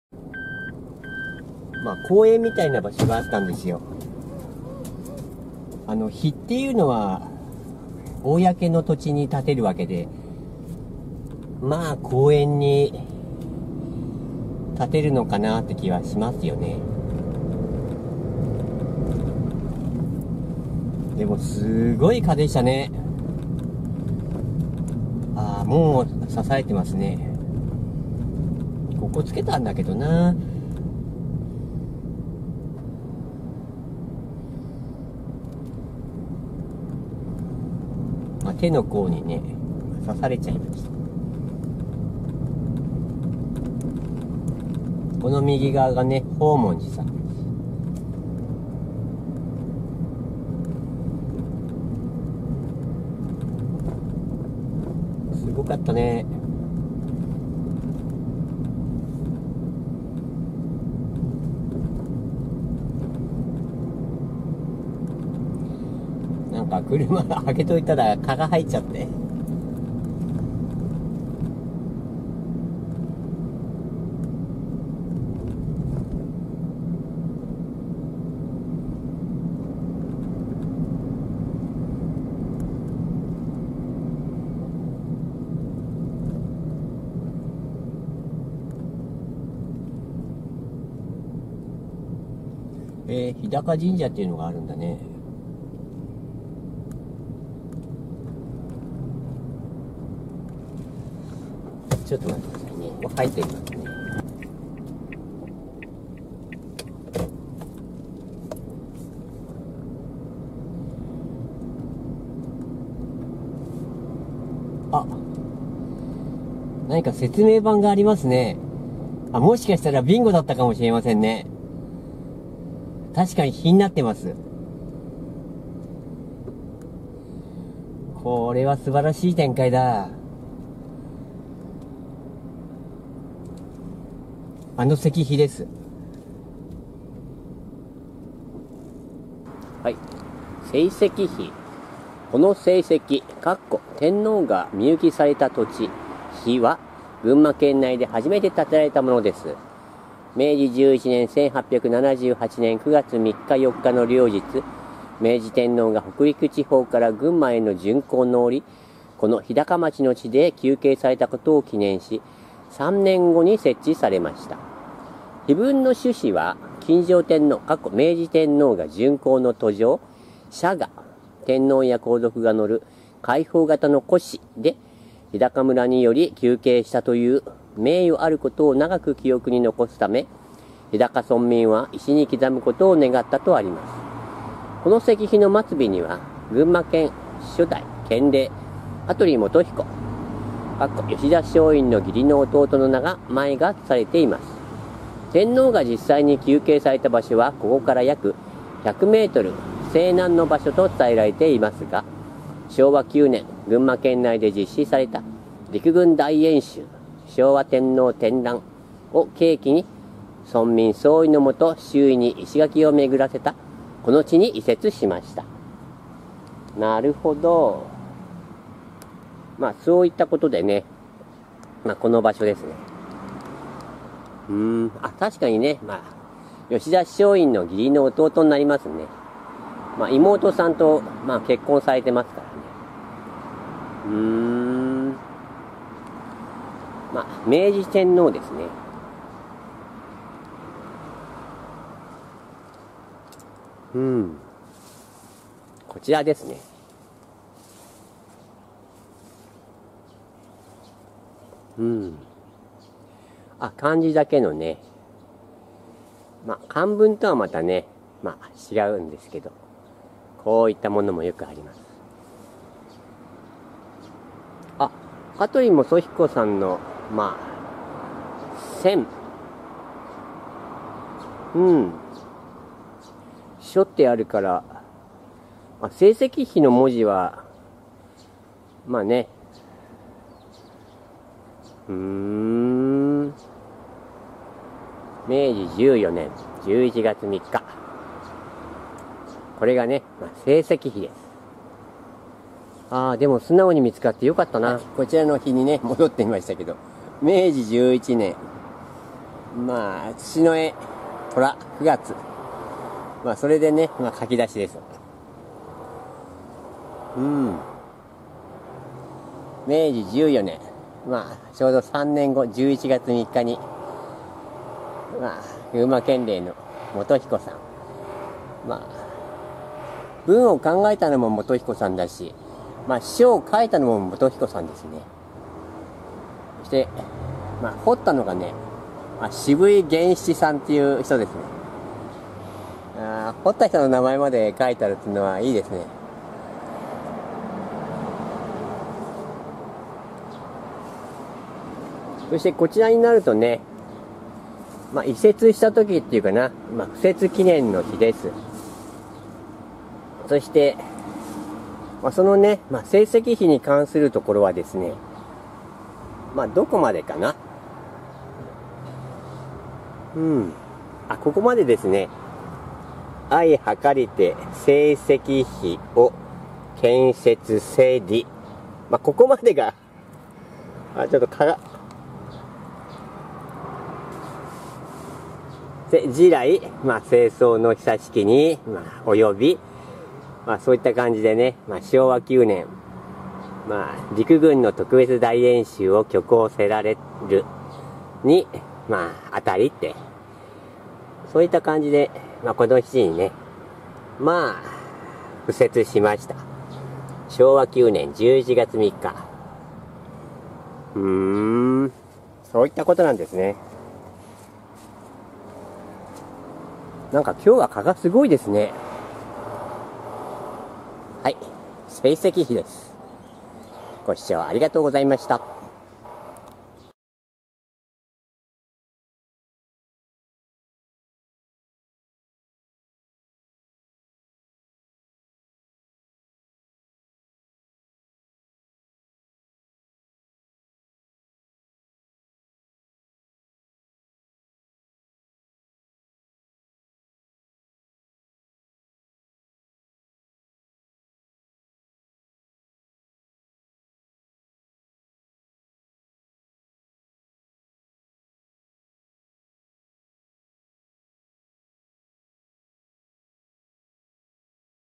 聖蹟碑・・高崎市日高町/群馬
楫取素彦・ゆかりの地マップに掲載されてます。小縮尺のマップなので、こんなに素直に見つかるとは思ってませんでした。ｗ